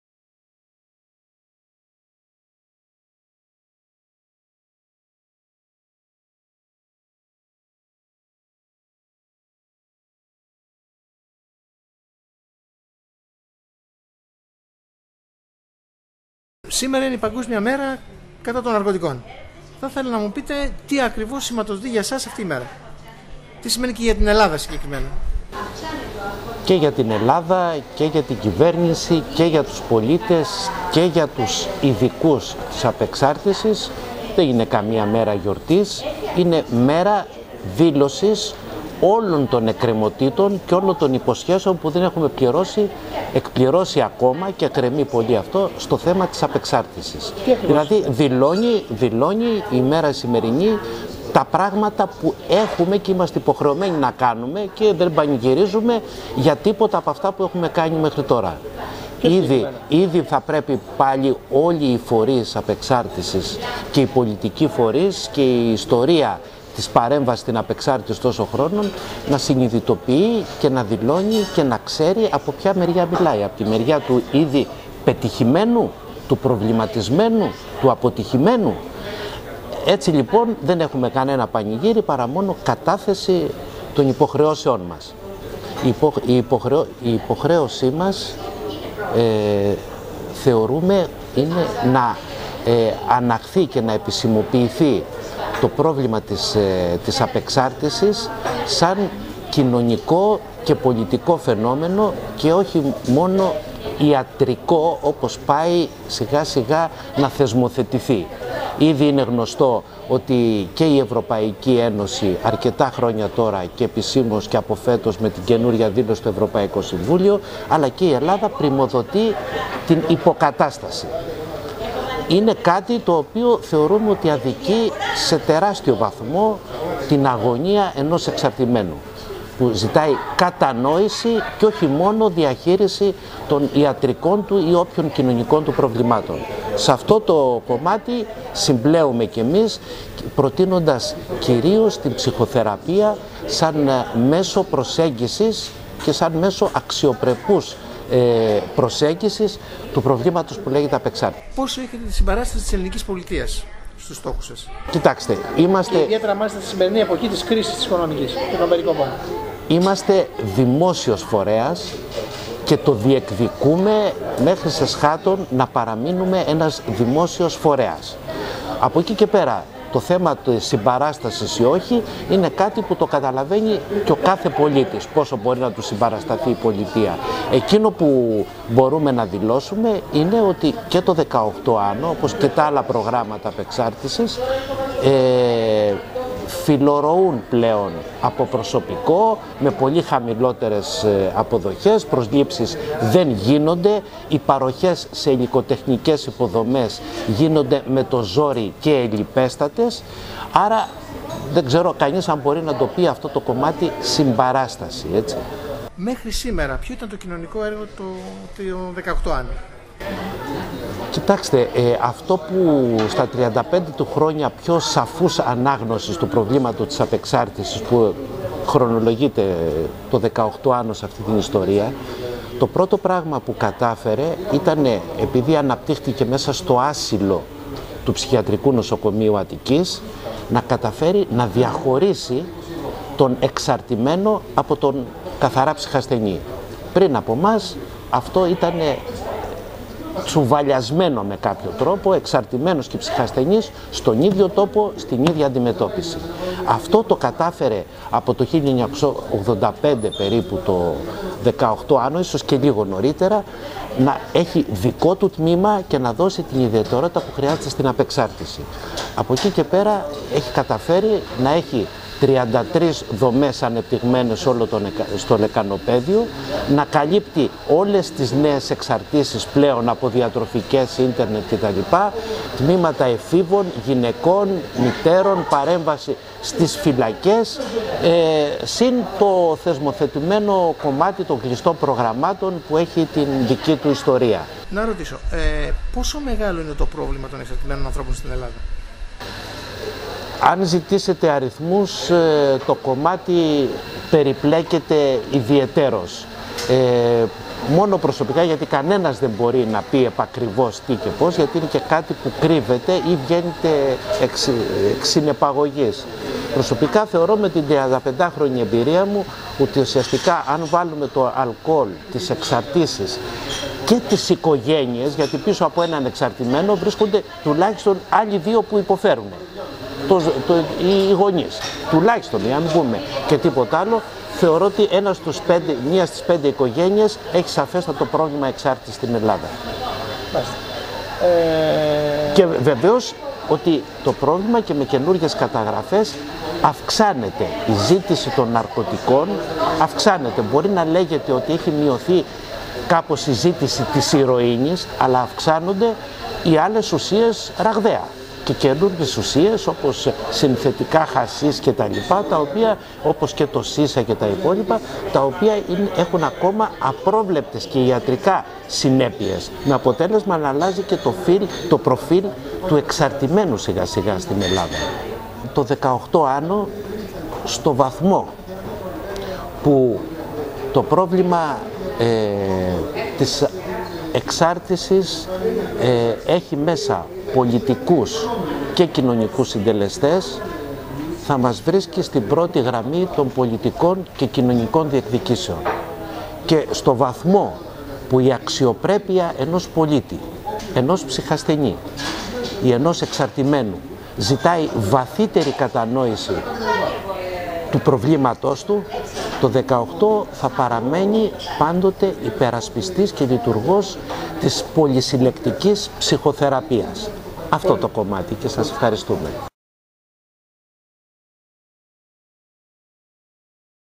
18 Άνω -Αλέκος Κυρούσης
Παγκόσμια Ημέρα κατά των Ναρκωτικών: συνέντευξη τύπου από το 18 Άνω
Ερωτήσεις-βίντεο: Άγγελος Καλοδούκας
Η Μονάδα Απεξάρτησης 18 Άνω με αφορμή την Παγκόσμια Ημέρα κατά των Ναρκωτικών διοργάνωσε συνέντευξη τύπου στην αίθουσα του βιβλιοπωλείου ΙΑΝΟΣ την Πέμπτη 26 Ιούνη. Το left ήταν φυσικά εκεί και πήρε δυο μίνι συνεντεύξεις. Από την Κατερίνα Μάτσα, ψυχίατρο, τέως διευθύντρια του 18 Άνω και τον Αλέκο Κυρούση, ψυχίατρο, διευθυντή στο πρόγραμμα απεξάρτησης 18 Άνω. Δείτε προσεκτικά αυτές τις δυο πολύ μικρές συνεντεύξεις (από 7 λεπτά η καθεμιά περίπου) και θα μάθετε πολλά για τις σκανδαλώδεις περικοπές στη σημερινή Ελλάδα που στοιχίζουν πλέον ανθρώπινες ζωές.